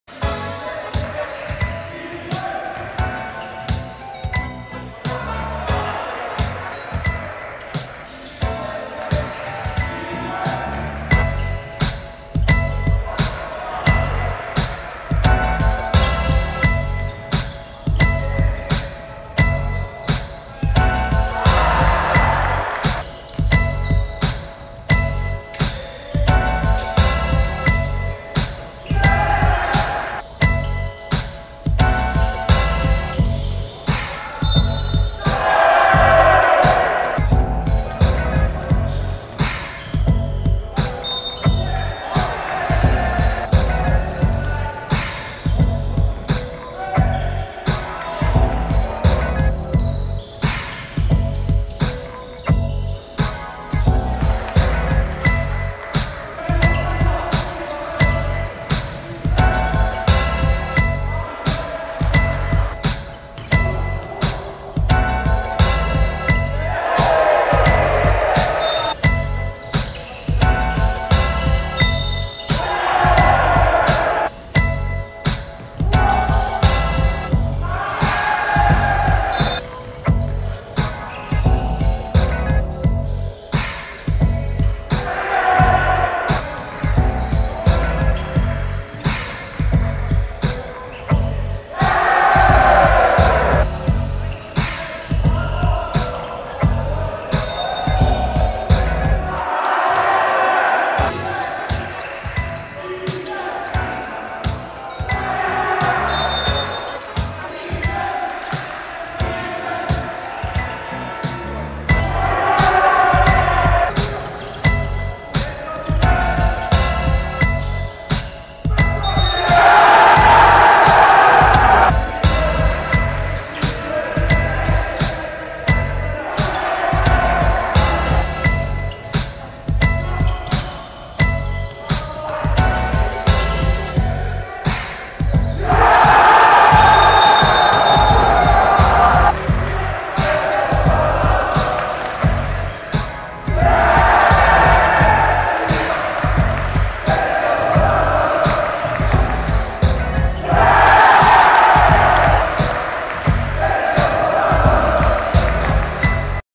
29 07 Półfinał Charlotte vs Los Angeles – Draft Camp 68
CHARLOTTE 67:65 LOS ANGELES po dogrywce
B.Mazur 24pkt 9-12fg 5zb 3ast
T.Solarczyk 26pkt 4x3 8-9ft